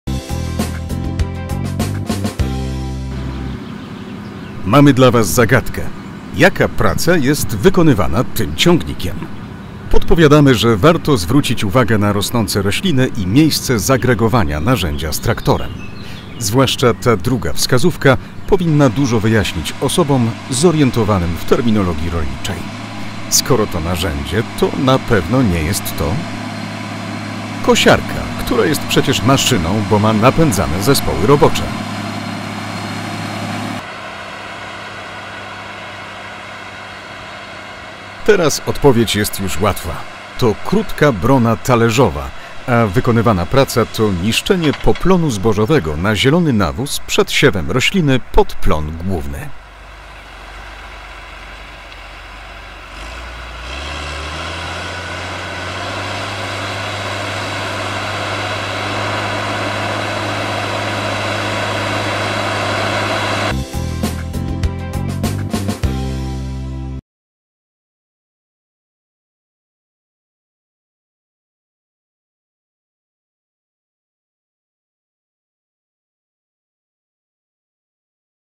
Zagadka - jaka to praca?
Mamy dla Was zagadkę dotyczącą techniki rolniczej.